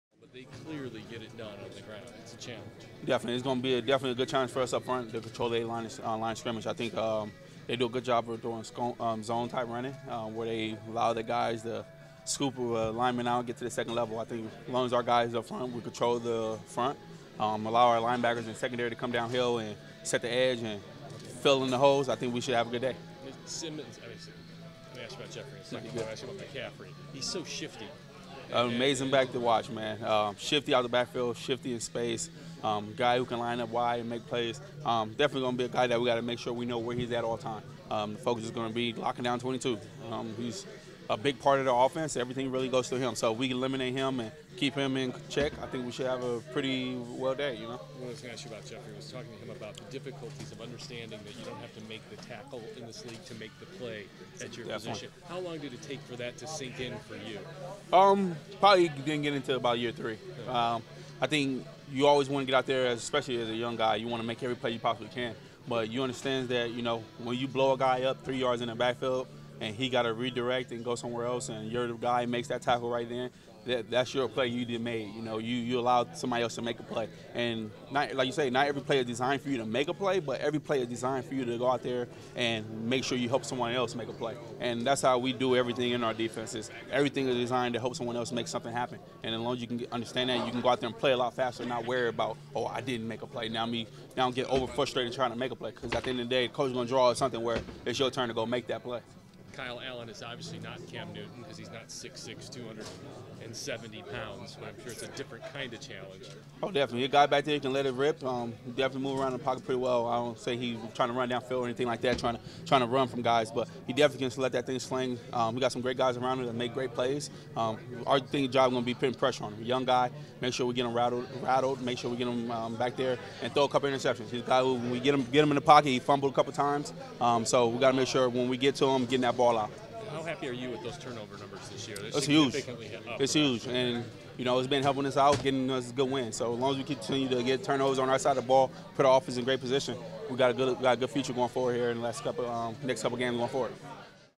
Jurrell Casey on Panthers QB Kyle Allen: He Can Sling It
Titans defensive tackle Jurrell Casey speaks with reporters Thursday at Saint Thomas Sports Park. The Titans take on the Carolina Panthers in Week 9 Sunday at Bank of America Stadium.